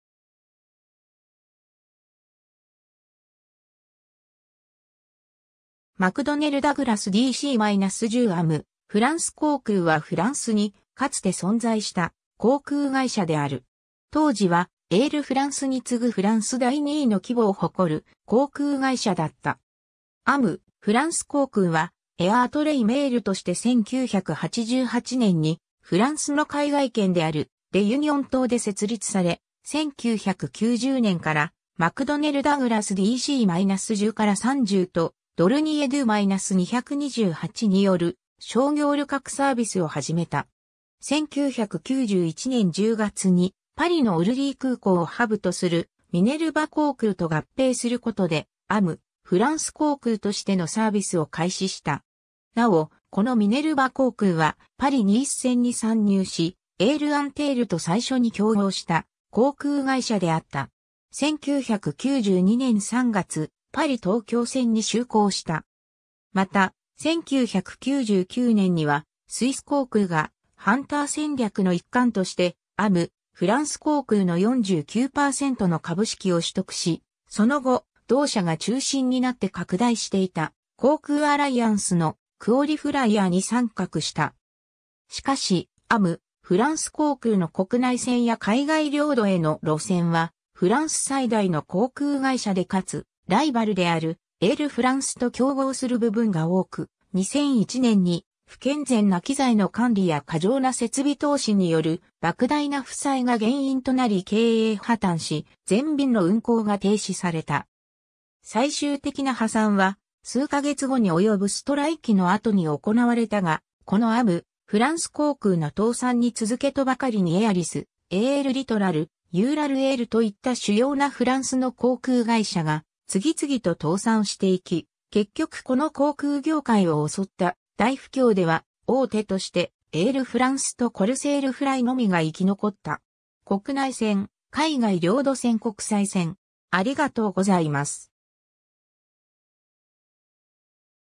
AOMフランス航空
マクドネル・ダグラス DC-10 AOMフランス航空（AOMフランスこうくう、AOM French Airlines）はフランスにかつて存在した航空会社である。
当時はエールフランスに次ぐフランス第2位の規模を誇る航空会社だった。
 AOMフランス航空はAir Outre Mer (AOM)として1988年にフランスの海外県であるレユニオン島で設立され、1990年からマクドネル・ダグラスDC-10-30とドルニエDo-228による商業旅客サービスを始めた。
 1991年10月に、パリのオルリー空港をハブとするミネルヴァ航空と合併することで、AOMフランス航空としてのサービスを開始した。
なお、このミネルヴァ航空はパリ（オルリー） - ニース線に参入し、エールアンテールと最初に競合した航空会社であった。
1992年3月、パリ（オルリー） - 東京（成田）線に就航した。
 また、1999年にはスイス航空が「ハンター戦略」の一環としてAOMフランス航空の49%の株式を取得し、その後、同社が中心になって拡大していた航空アライアンスの「クオリフライヤー」に参画した。
 しかし、AOMフランス航空の国内線や海外領土への路線はフランス最大の航空会社でかつ、ライバルであるエールフランスと競合する部分が多く、2001年に不健全な機材の管理や過剰な設備投資による莫大な負債が原因となり経営破綻し、全便の運航が停止された。
 最終的な破産は数カ月後に及ぶストライキの後に行われたが、このAOMフランス航空の倒産に続けとばかりにエアリス、エール・リトラル、ユーラルエールといった主要なフランスの航空会社が次々と倒産していき、結局この航空業界を襲った大不況では、大手としてエールフランスとコルセールフライのみが生き残った。
 国内線・海外領土線 国際線